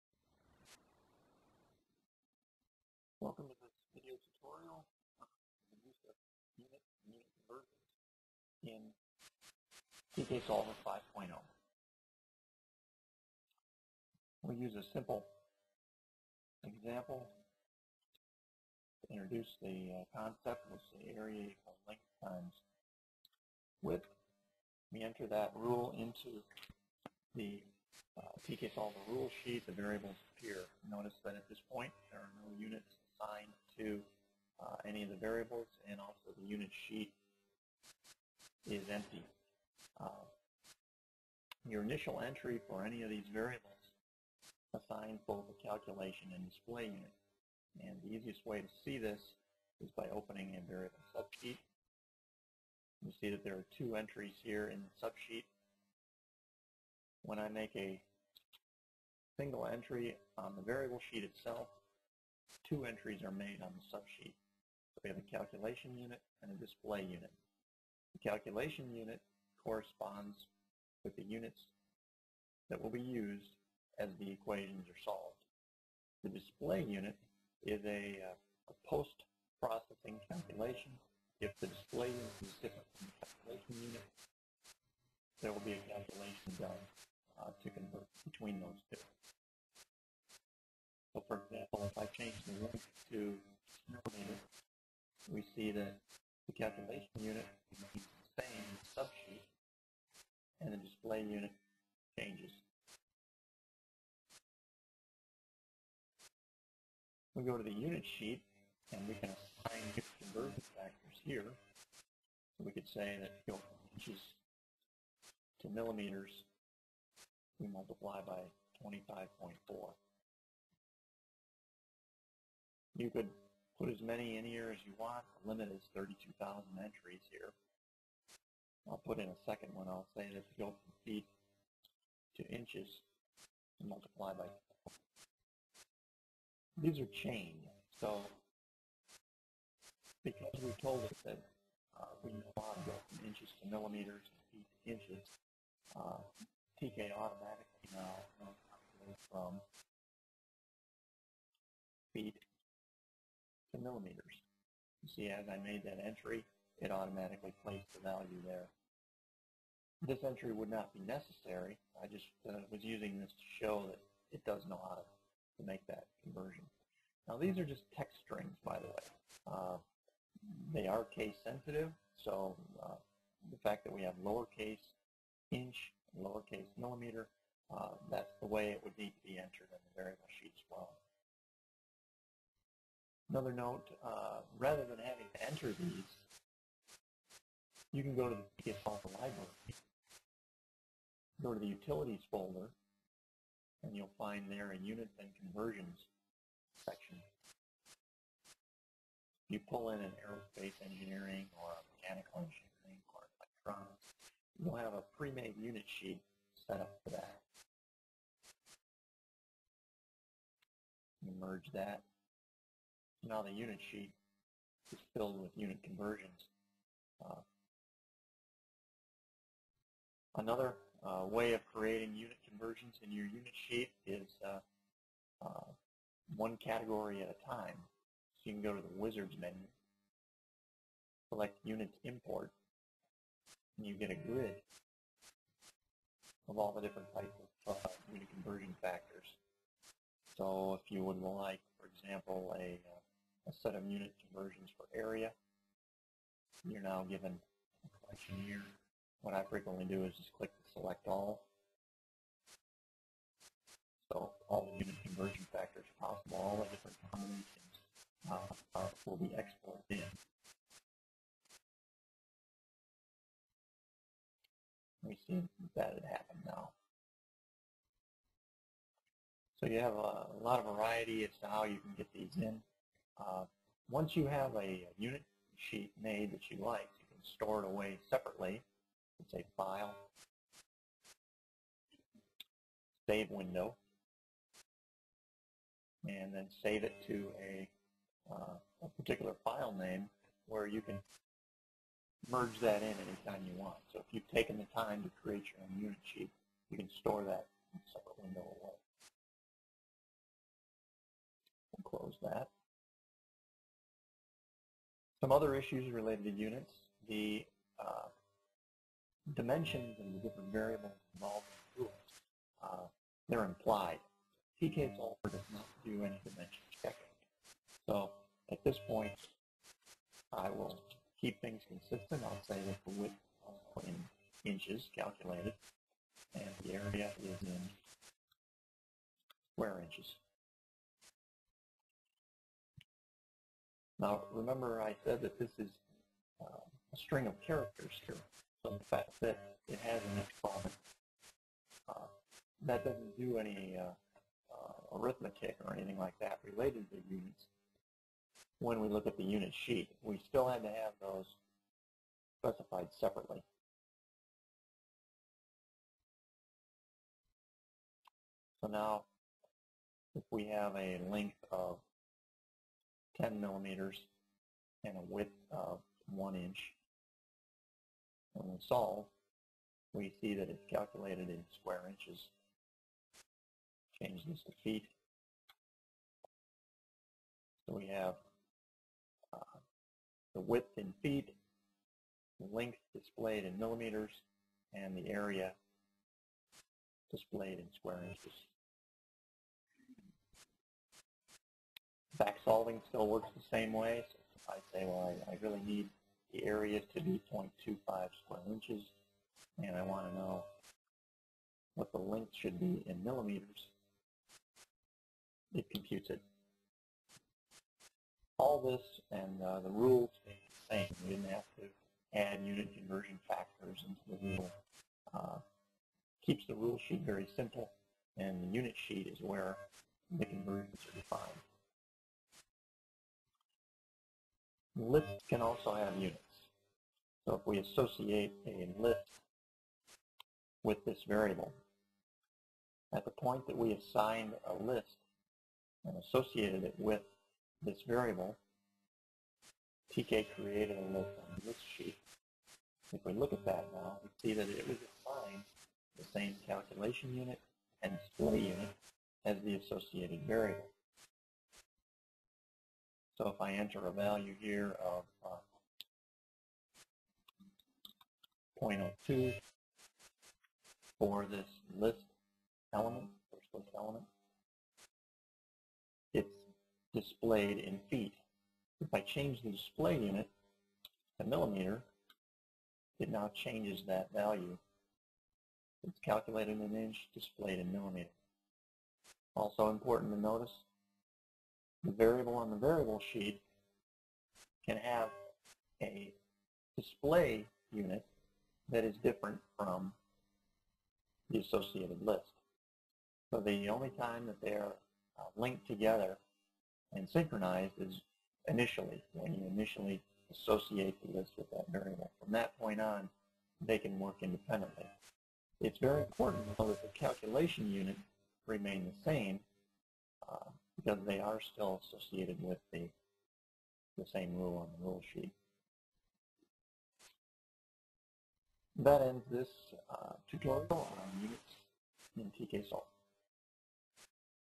TK Solver - Unit Conversions
This clip shows how you can convert between units of your choice for your math models-it's easy to do in TK Solver.